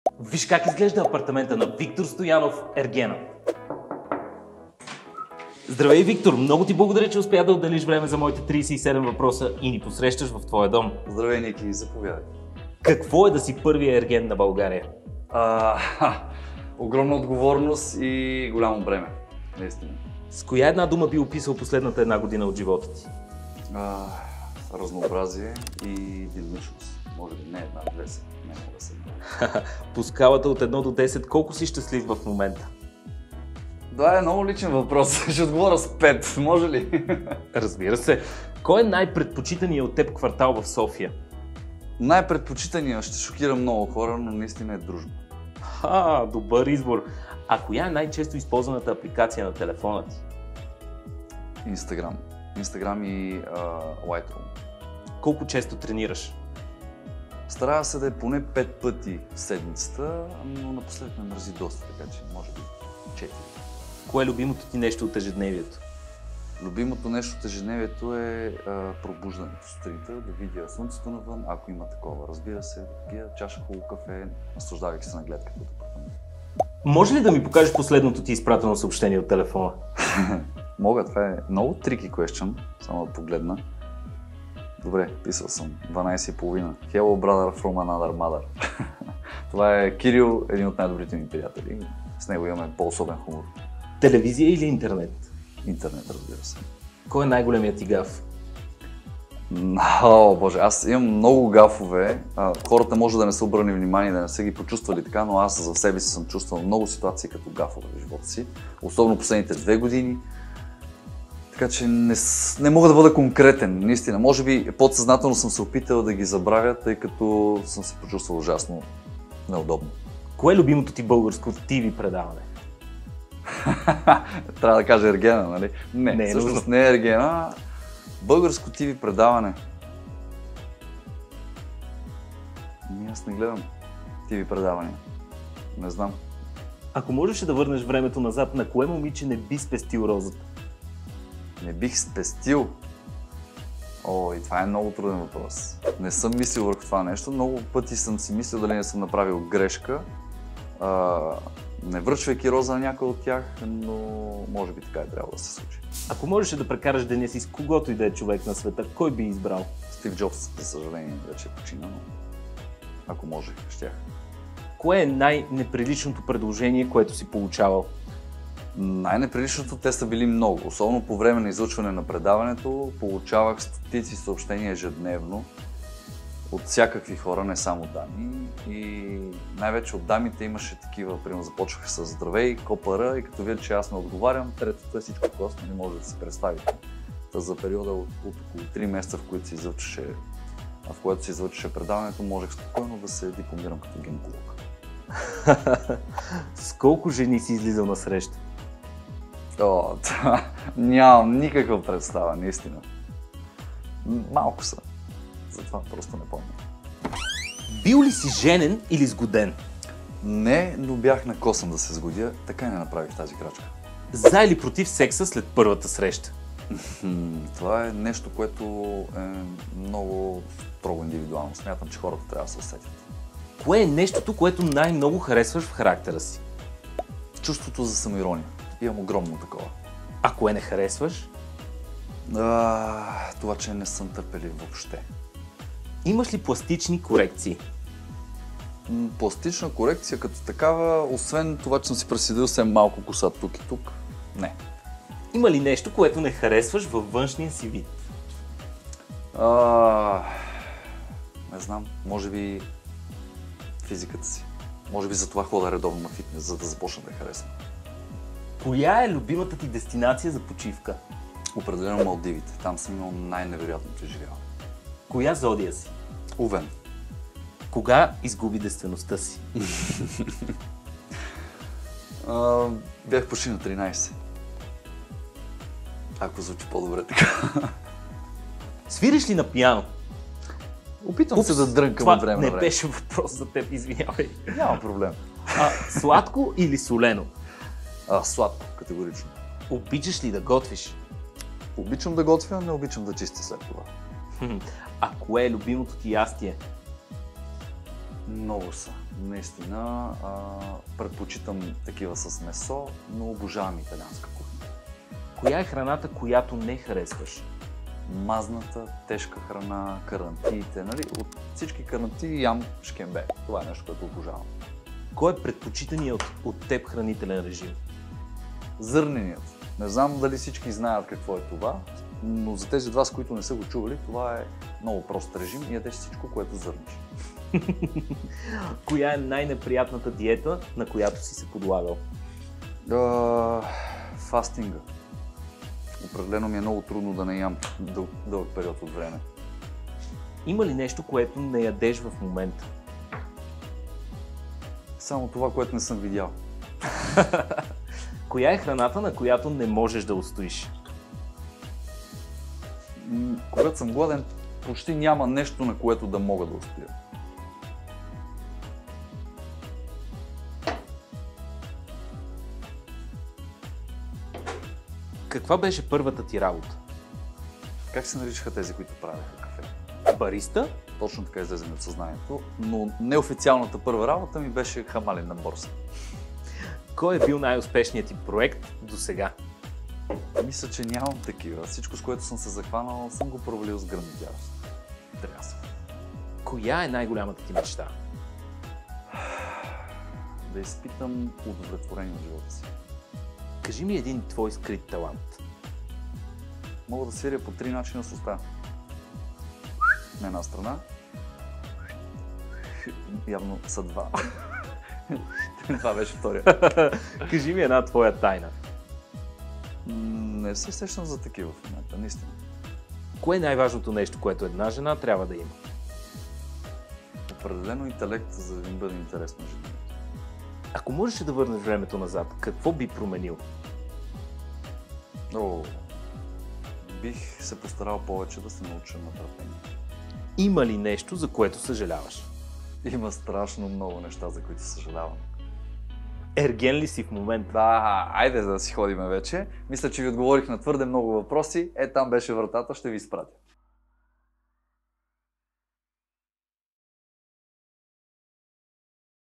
37 ВЪПРОСА в дома на ВИКТОР СТОЯНОВ - ЕРГЕНЪТ
Докато разглеждам апартаментa в който живее Виктор Стоянов, му задавам моите 37 въпроса и си говорим за жени, рози и още нещо.